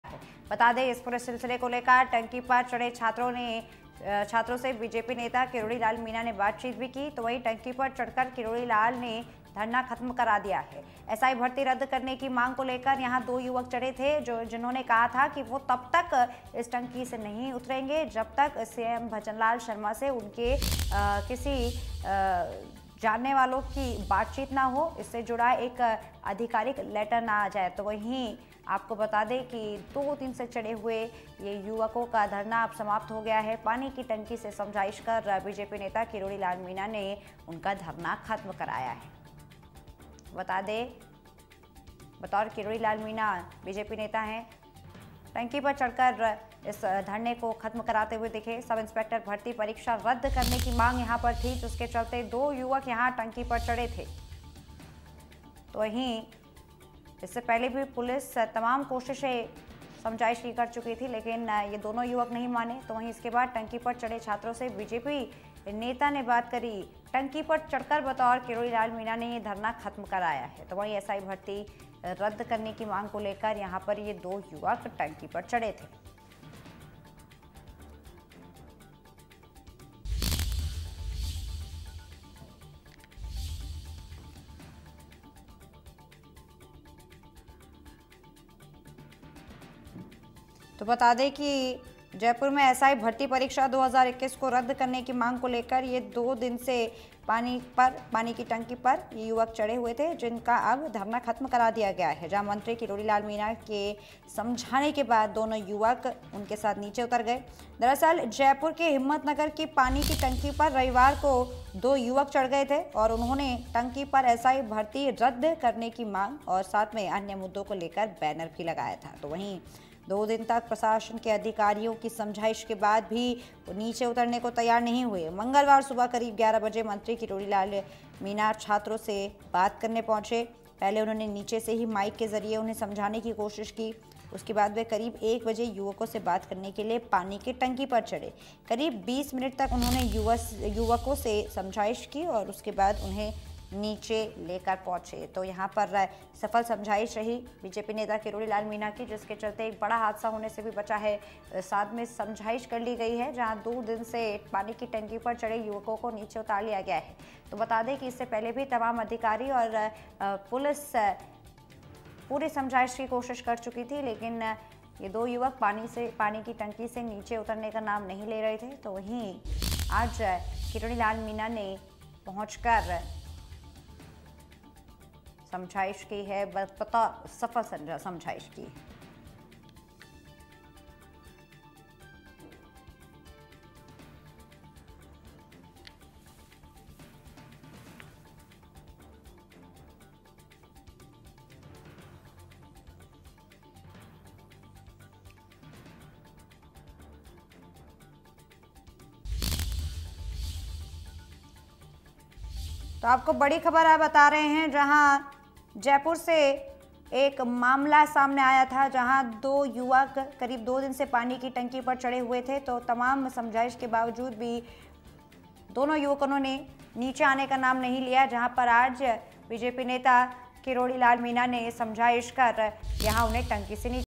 Kirodi Lal Meena के कहने पर पानी की टंकी पर चढ़े युवकों ने किया धरना रद्द | India News Rajasthan
राजस्थान में SI भर्ती परीक्षा 2021 को रद्द कराने को लेकर सरकार अभी विचार विमर्श कर रही है. जिसके बाद कुछ युवा परीक्षा रद्द करने के पक्ष में प्रदर्शन भी कर रहे हैं. ऐसे में राजस्थान सरकार के सामने भी भी SI परीक्षा को रद्द करने और न करने की विकट समस्या है. लेकिन दूसरी ओर परीक्षा रद्द करने की मांग को लेकर कुछ युवा अग्र दिख रहे हैं. किरोड़ी लाल मीणा ने विद्यार्थियों से बात कर के उनका प्रदर्शन ख़तम करवाया और उनको शांत होने के लिए कहा.